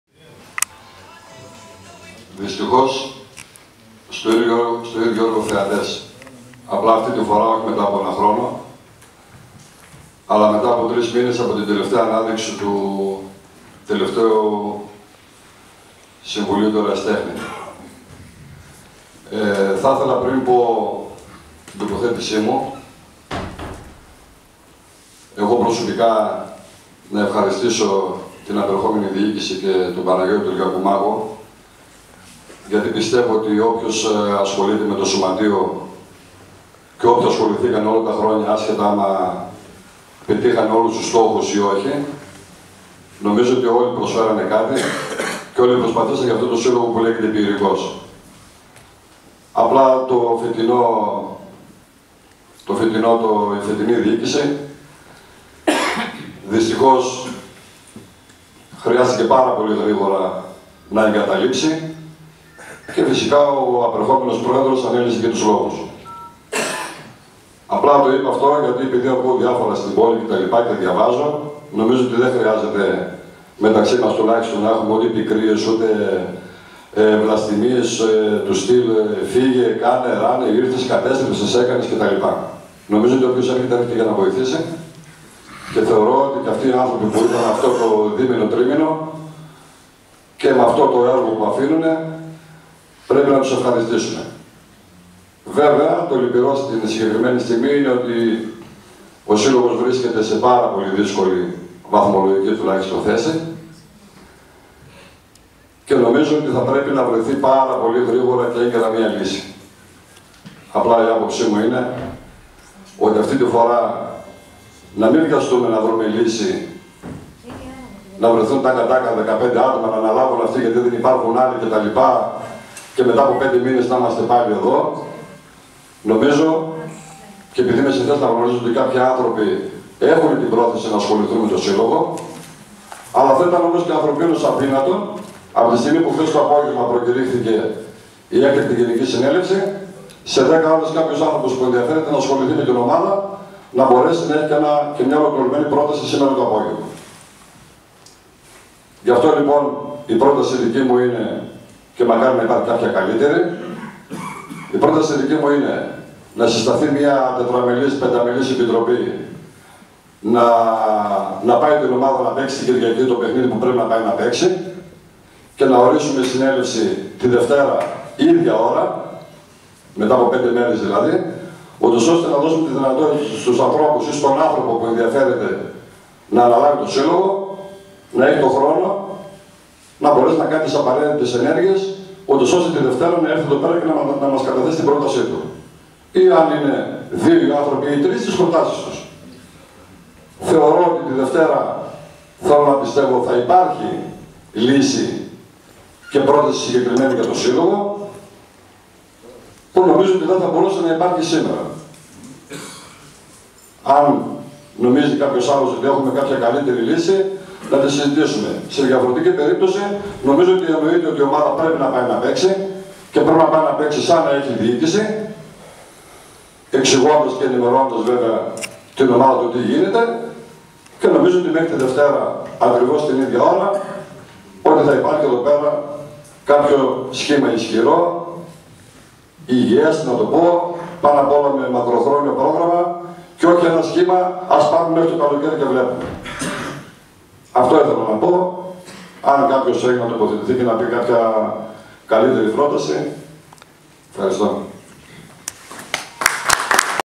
katerinisport.gr: Τοποθέτηση Τσιτιρόκη | ΣΦΚ Πιερικός - Έκτακτη Γ.Σ. (14-11-18) 2/14
Δείτε την τοποθέτηση του πρώην Προέδρου του ΣΦΚ Πιερικός και μέλους του συλλόγου, Πασχάλη Τσιτιρόκη, στην Έκτακτη Γ.Σ. του σωματείου, το απόγευμα της Τετάρτης 14 Νοεμβρίου 2018, στην "Εκάβη". Η κάμερα της webtv του katerinisport.gr ήταν εκεί.